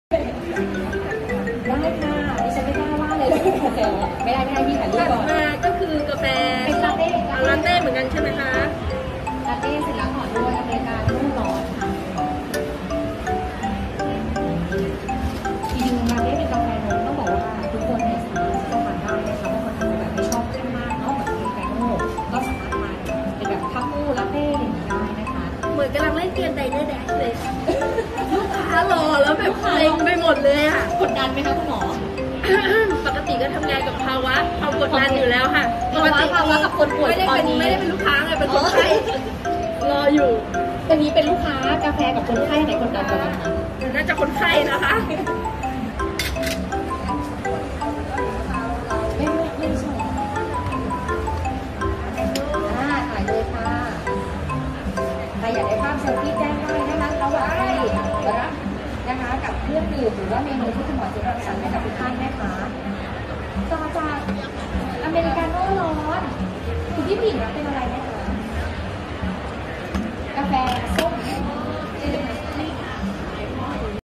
หมอเจี๊ยบลลนา​ วันนี้มาในมาด|บารีสต้าสุดหล่อ|Boncafe|BoncafeThailand​jeab_lalana​|25.12.64
วันนี้ใครได้ชิมฝีมือการทำกาแฟสด​จากหมอนี้สุดยอด​ไปเลยจ้า25.1264